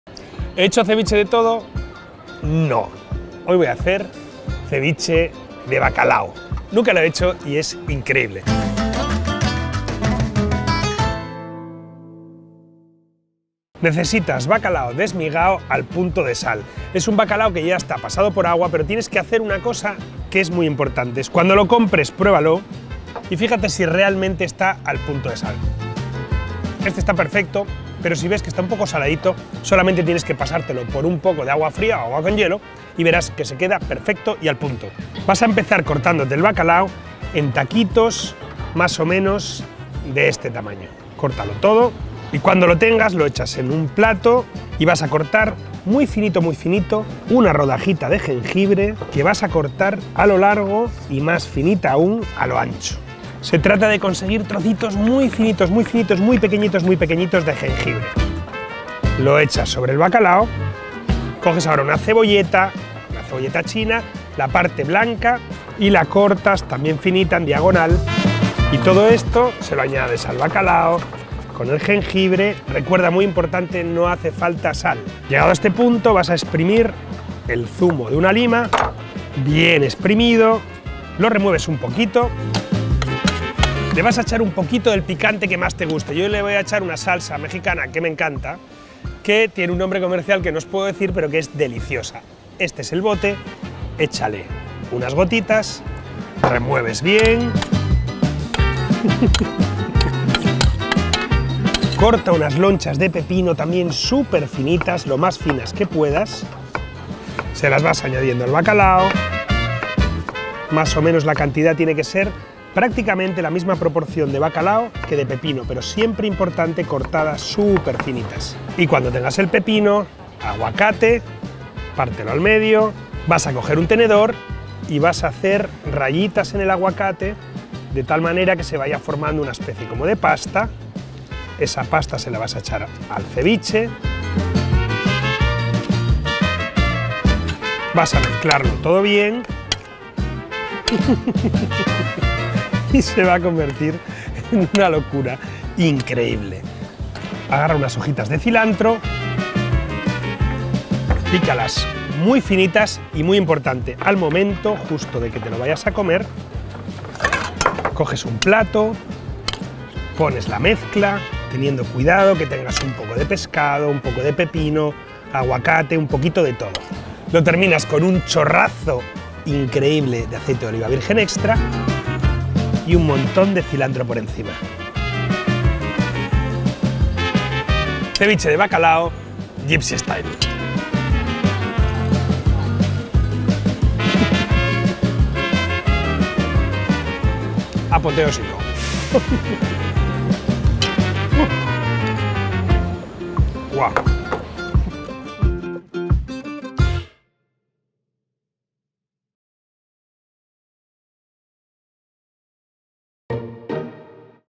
Receta refrescante de CEVICHE de BACALAO | Gipsy Chef
Queridas, queridos, el ceviche lo aguanta todo o casi todo. Qué razón tenía que, sin saberlo, ya sabía que era increíble!! Pues ahora ya lo sé, este ceviche es una joyita. De Perú a Noruega pasando por Barcelona. Ahí va uno de los cross-overs más deliciosos del planeta.

(Coproducción Gipsy Chef/La Vanguardia/grupo Godó)-— @comerlavanguardia